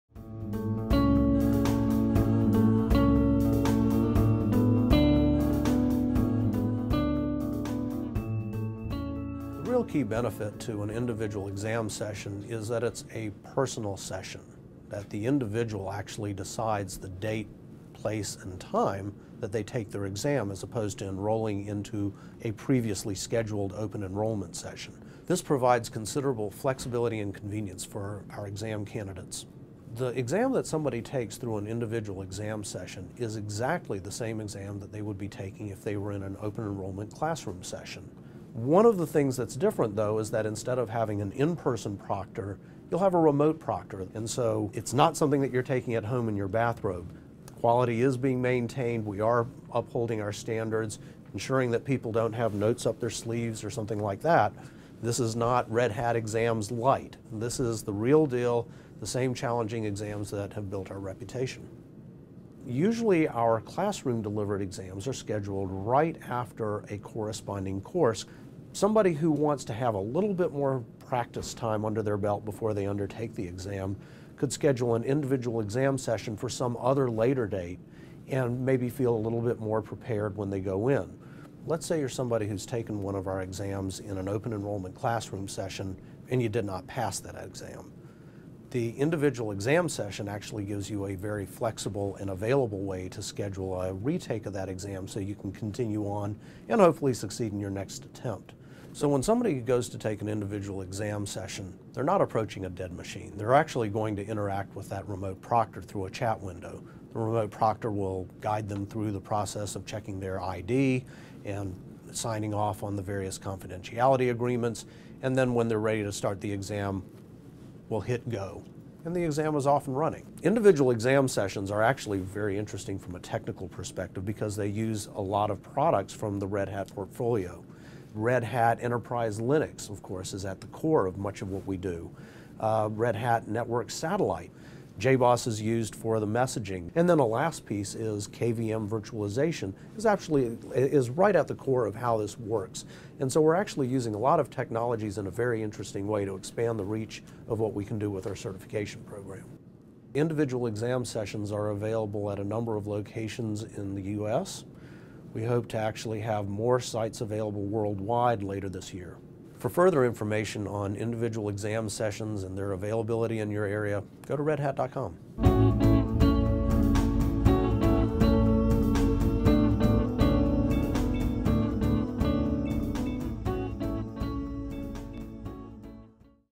Red Hat Training: Individual Exam Sessions
Individual Exam Sessions, alternatives to the traditional classroom environment, are now available through personal testing stations and allow customers more choice in taking and scheduling many of Red Hat's most popular exams.

At Red Hat, we are continually seeking new ways to provide customers and developers who are interested in becoming Red Hat certified with new, more flexible ways to take exams. To that end, Red Hat Training is proud to announce an innovative new exam delivery method that provides candidates with greater convenience.